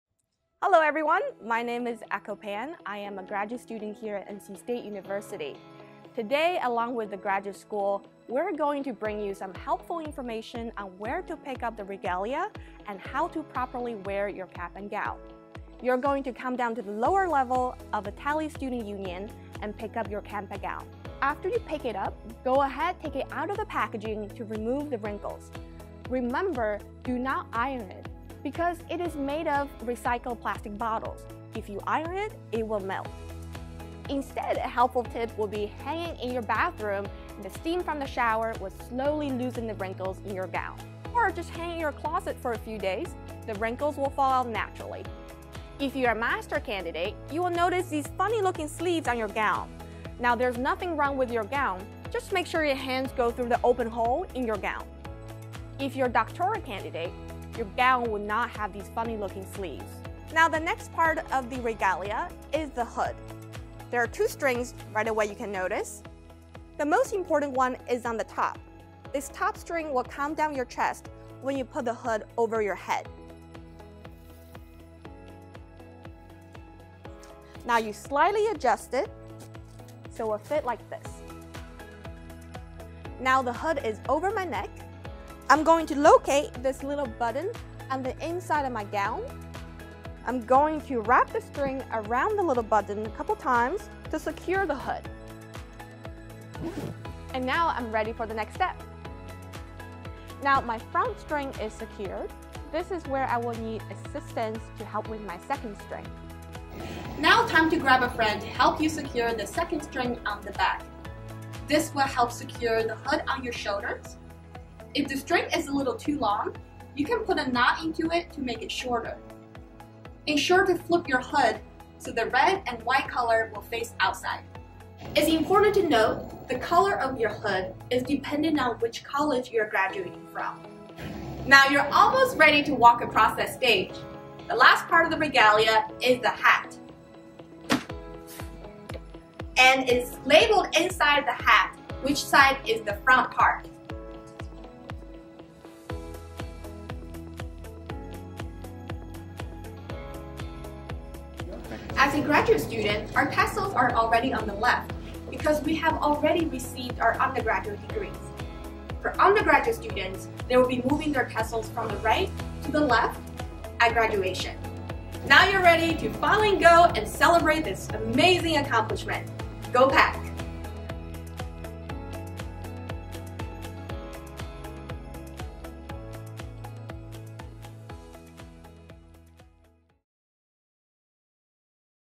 NC State Graduation Regalia Tutorial
Graduate student Echo Pan demonstrates how to properly wear graduate student regalia and shares some helpful tips on how to keep it wrinkle-free. A special thank you to Margaret Baker, PhD Student, Communication, Rhetoric & Digital Media and Wolfpack Outfitters.